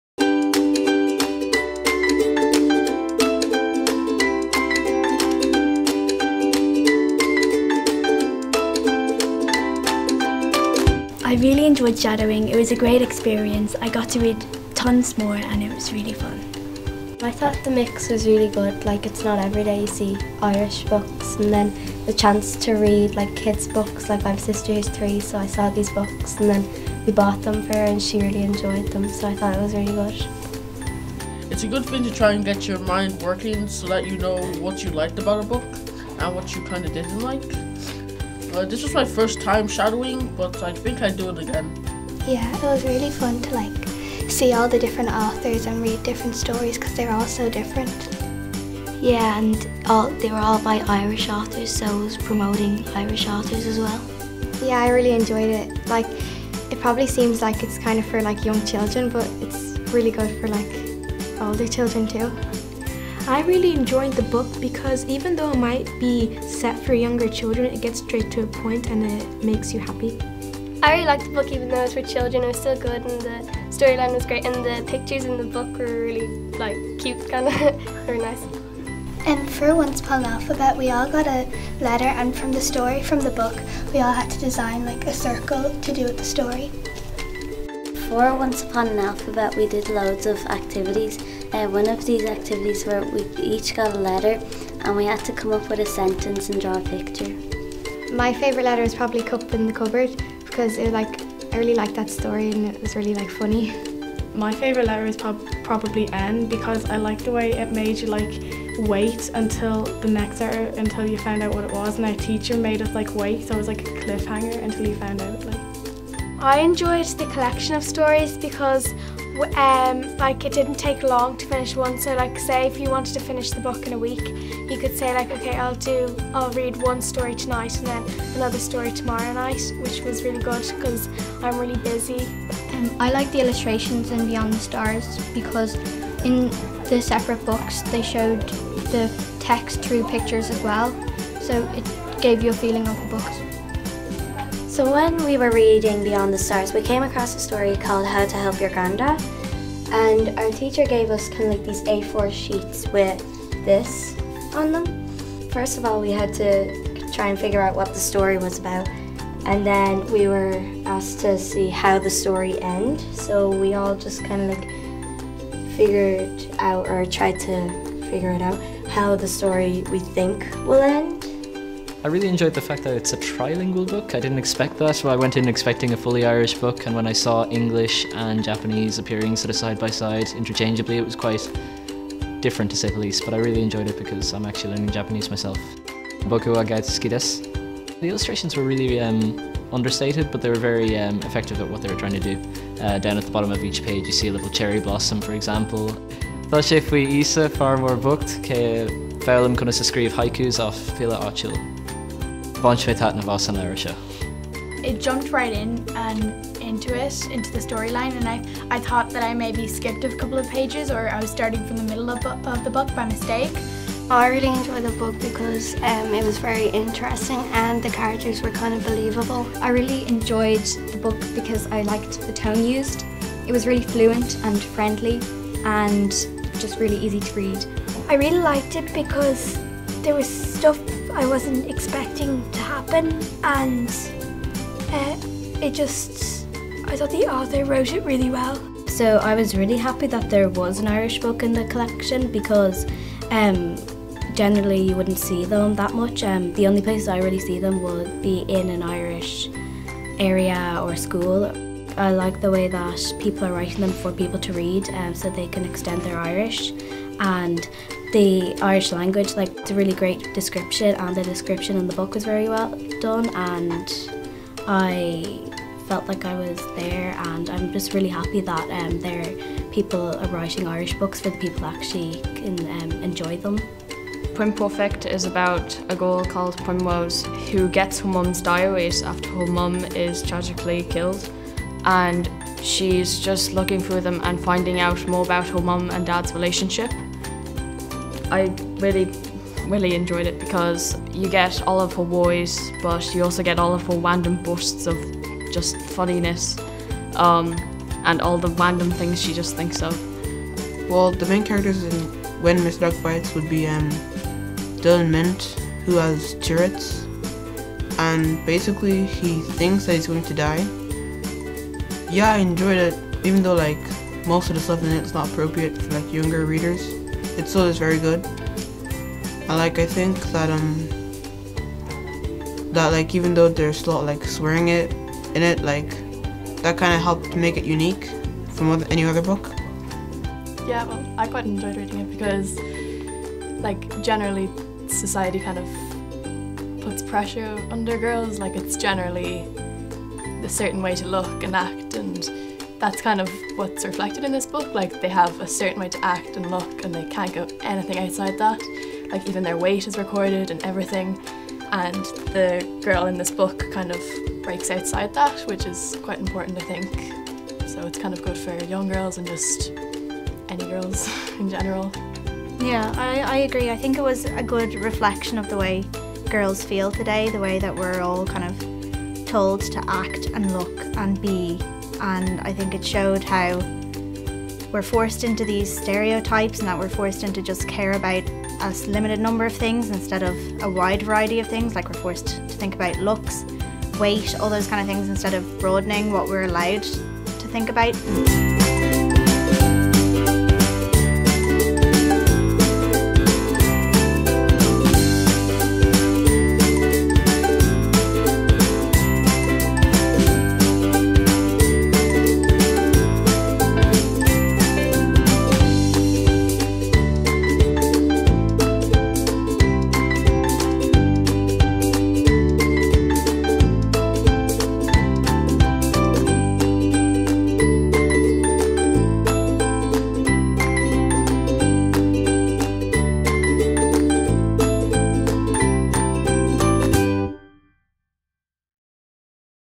CBI Book of the Year Awards Shadowing Video 2015
Go behind the scenes with CBI's Book of the Year Awards Shadowing Groups to find out what they thought of the 2015 shortlisted titles.